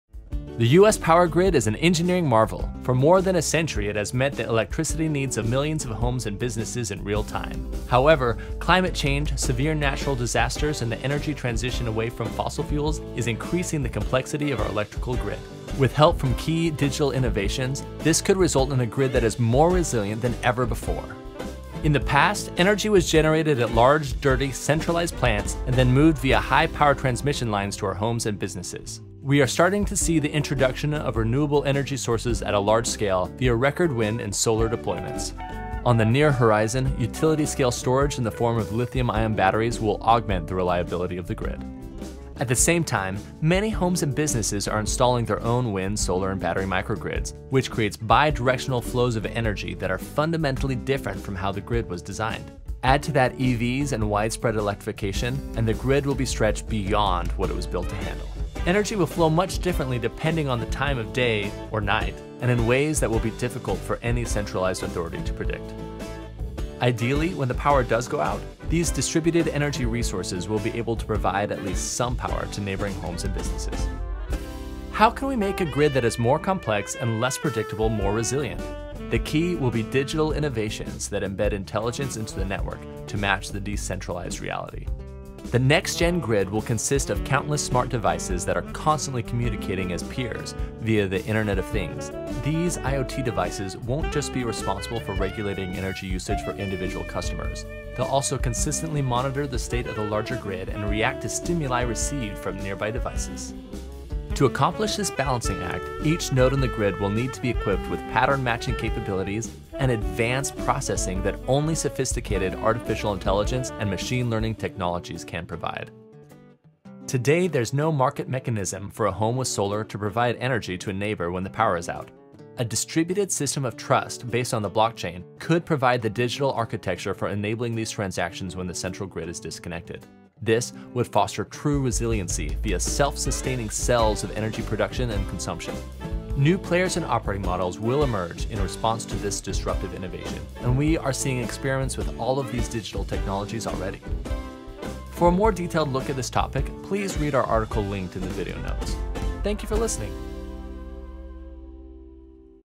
Microgrids, Blockchain and the Future of Decentralized Energy
The centralized hub-and-spoke model that has defined the U.S. power grid throughout its existence is nothing short of an engineering marvel. For more than a century, it has allowed utilities to distribute electricity to homes and businesses across the continent, and today it delivers energy to nearly every American citizen around the clock. However, the system is far from perfect.

Thanks to the improved economics of renewable energy and rising interest in residential solar power, microgrids are emerging as a viable alternative to the centralized power generation and distribution model.

For a more detailed look at this topic, read the full article